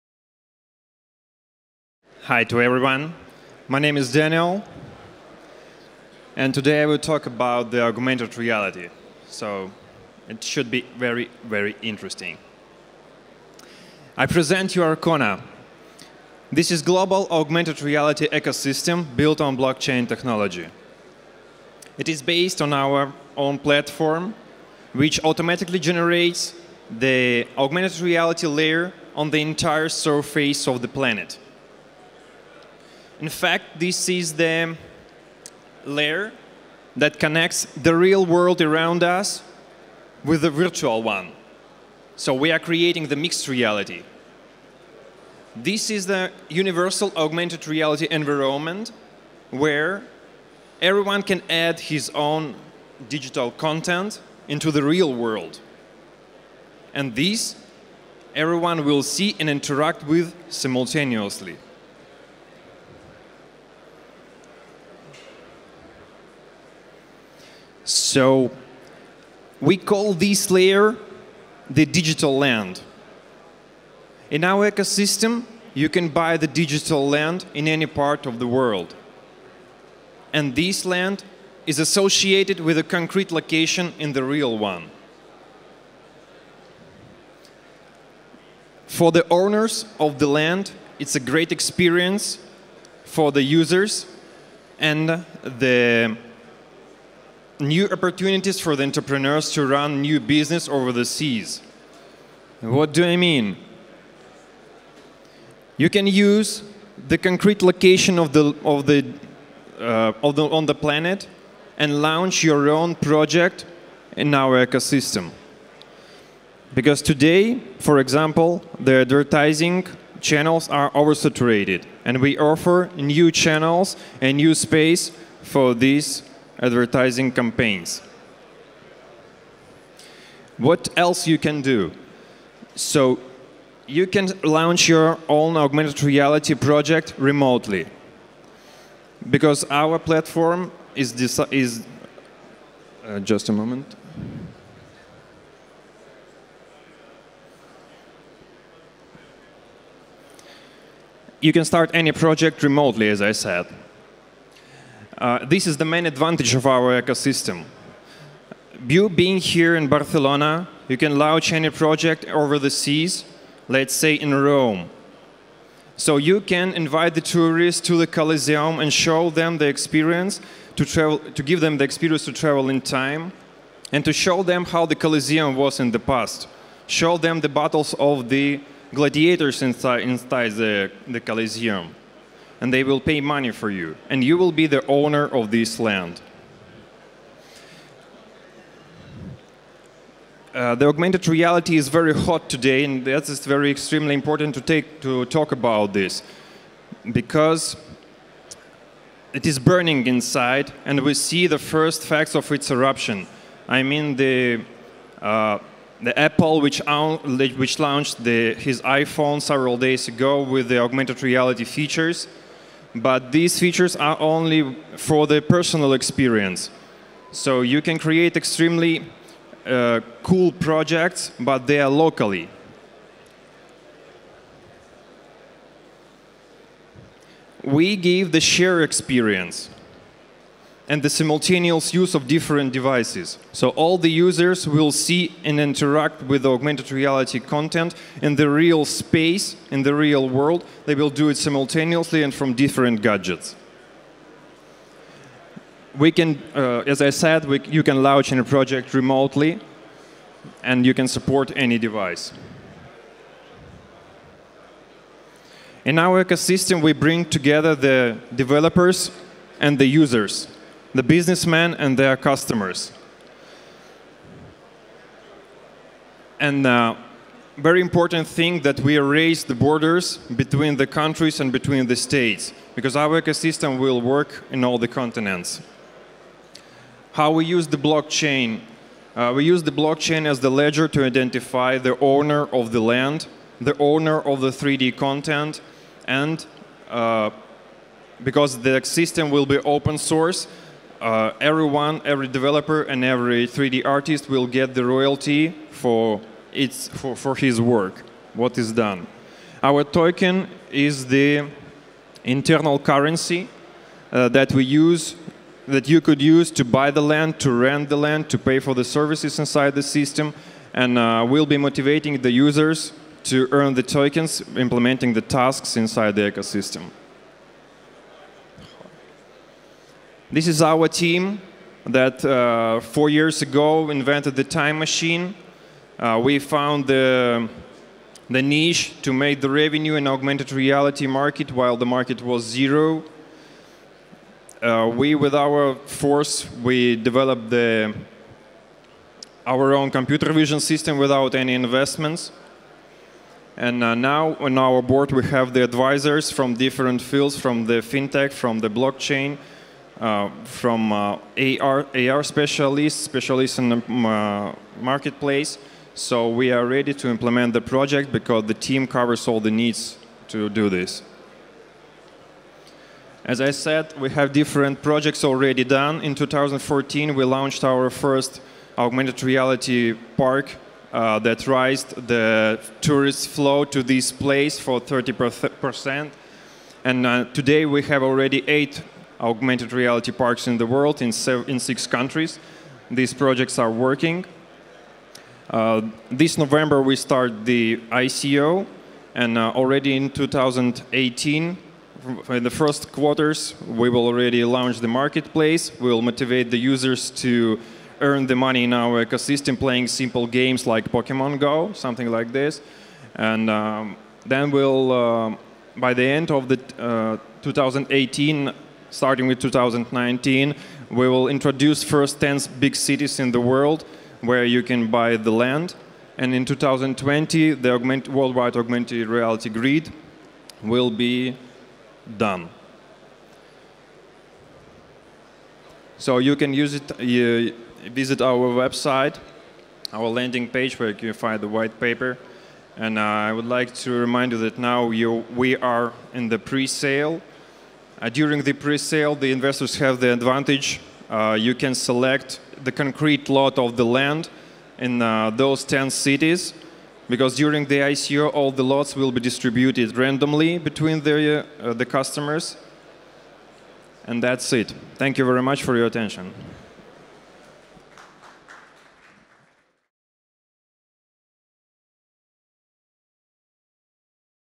Daniil Girdea - about Arcona Augmenter Reality Ecosystem
The Arcona Ecosystem creates a Digital Land - a layer of Augmented Reality uniting the physical and virtual worlds into a single information environment perfectly linked to our real world. It is designed for everyday user interactive experience with augmented, virtual and mixed reality multimedia content in real world locations.